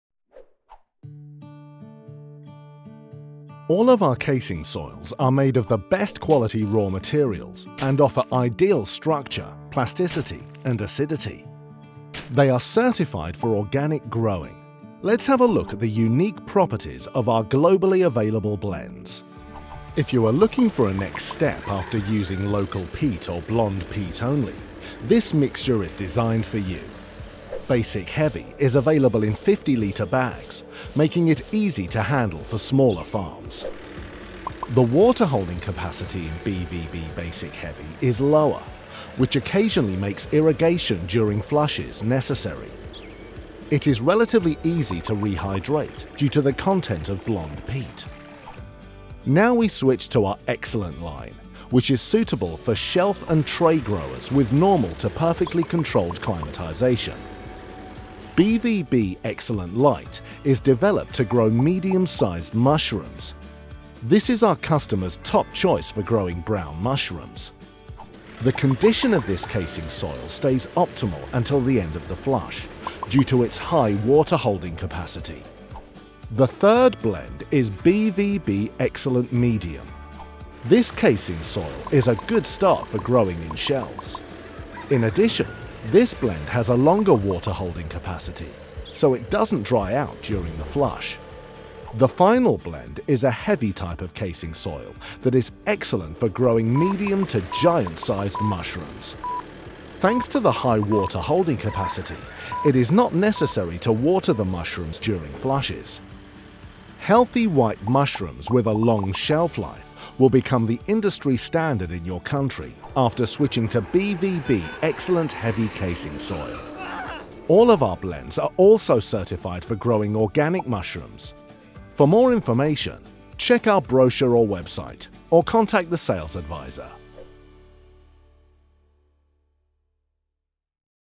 Different types of Mushroom Casing Soil
In this video we show the different properties and uses of our mushroom casing soil. Contact our experts if you'd like to know more: